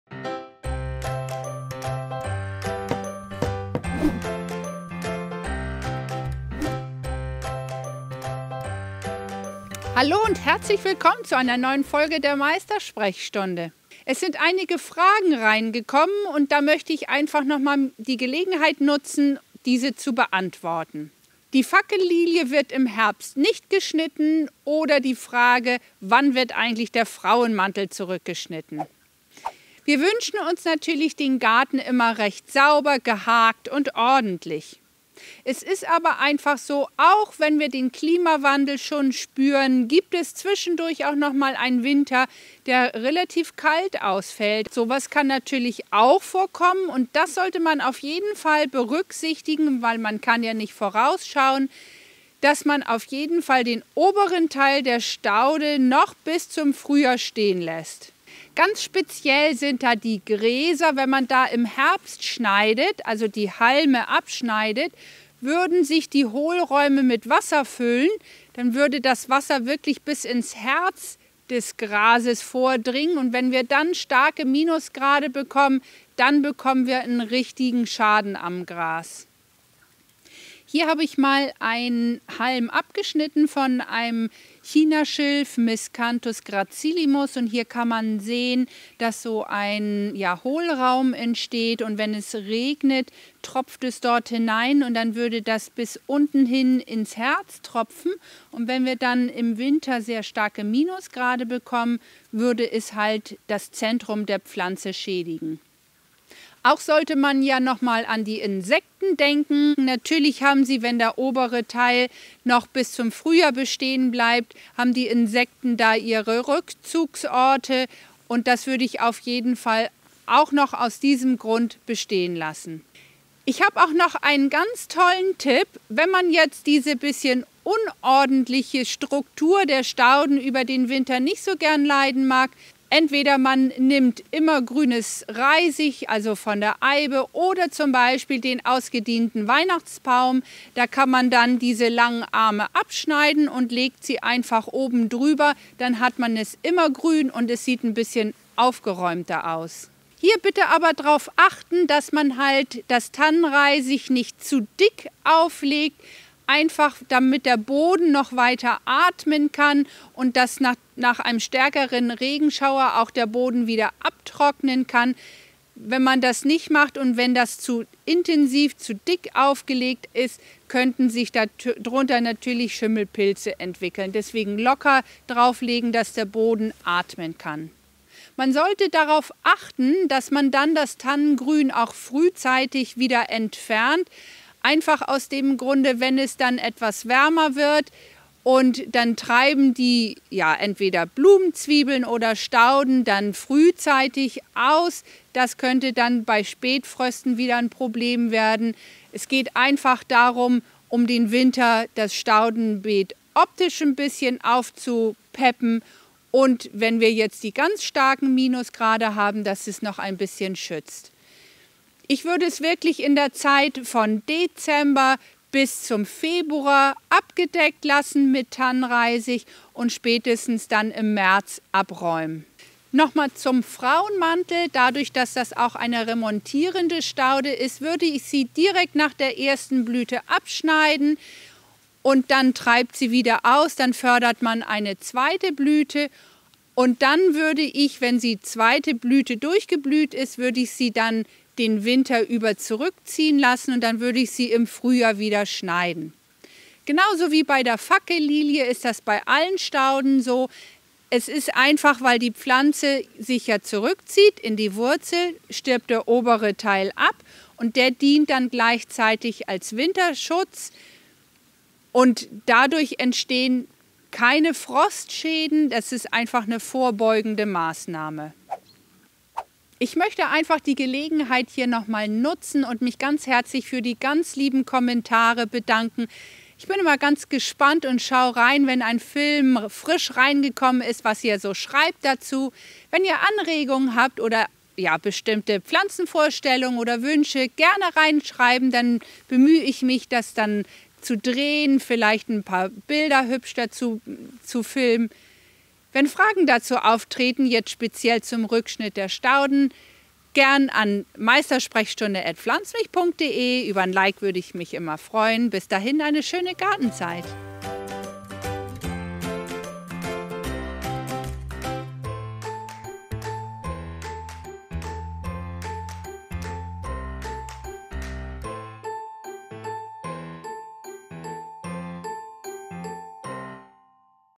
Tipps zur Überwinterung von Stauden
Es gab einige Fragen zum Rückschnitt von Stauden, die Ruth zum Anlass nimmt in dieser Folge nochmal einige Tipps zur richtigen Überwinterung von Stauden zu geben.